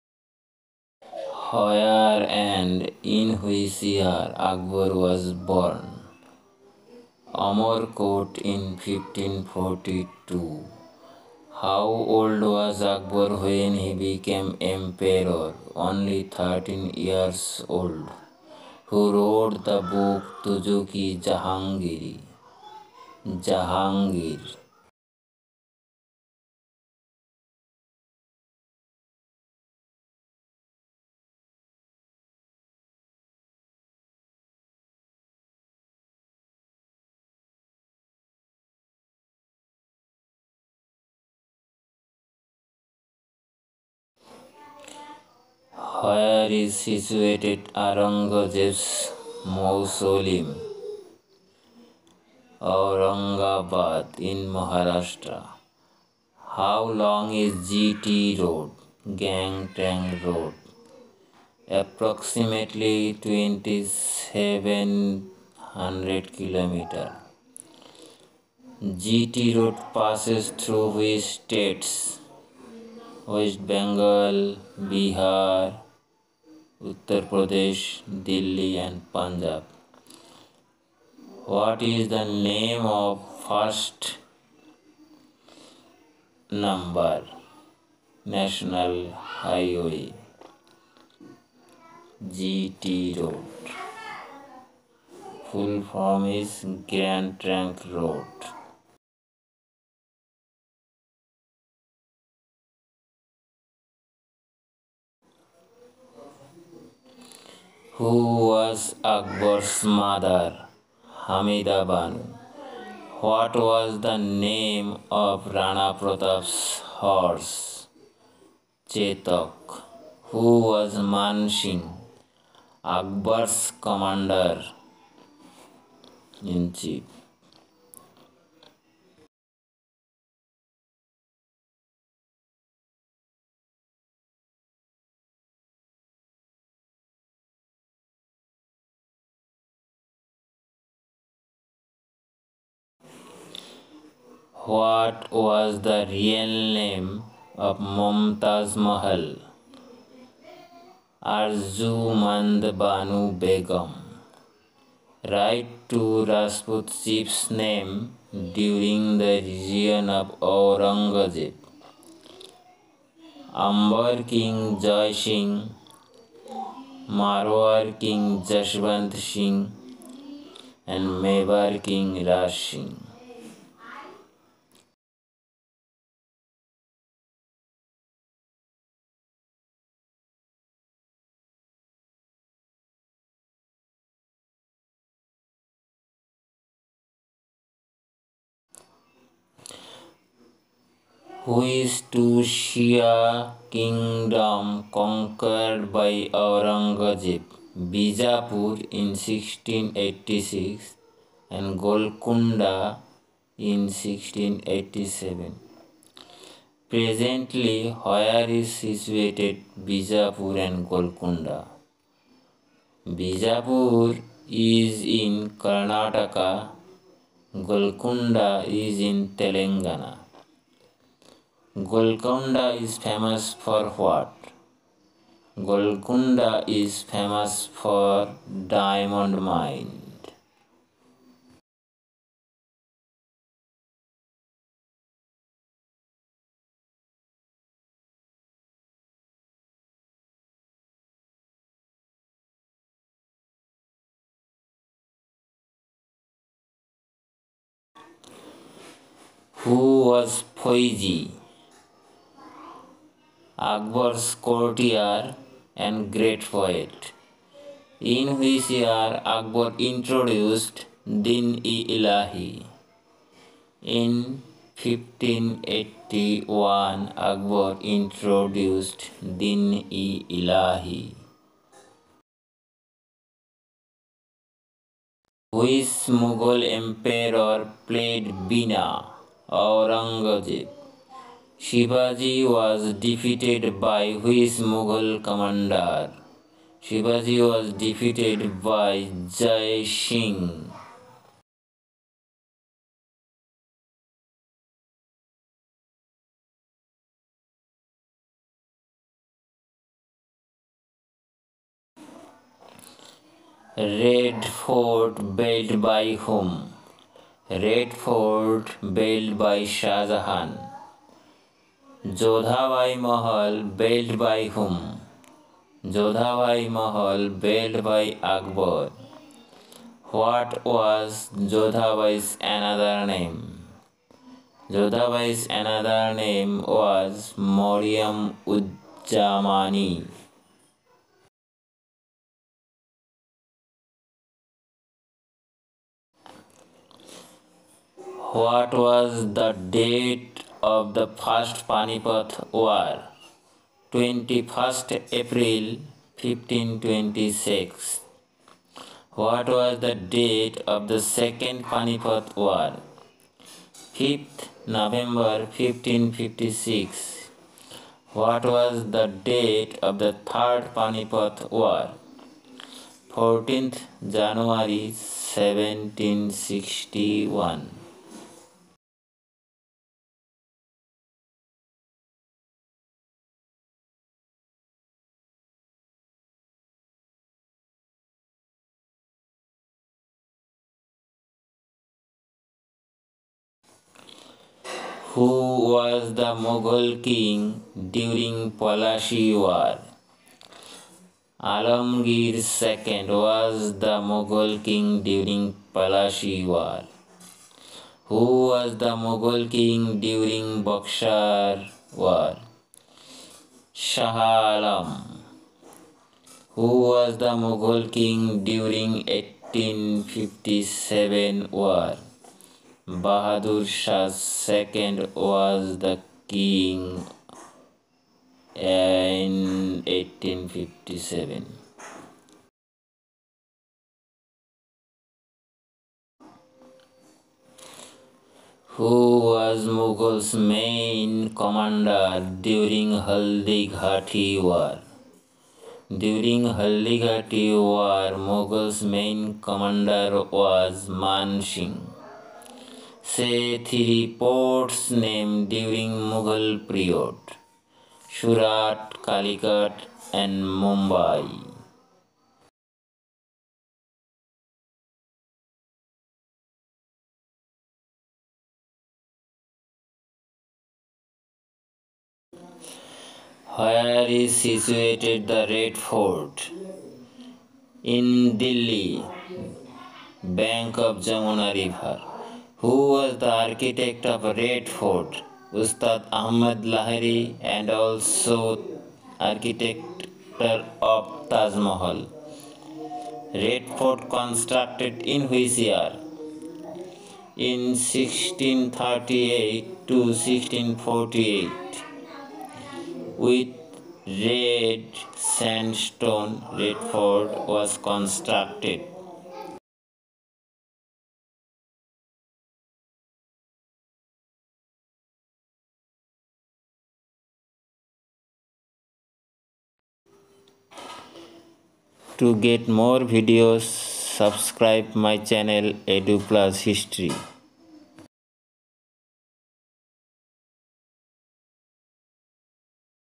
Mughal history!History Subject Gk
This educational video is every govt
exam related Gk Questions and Answers in history subject in english
language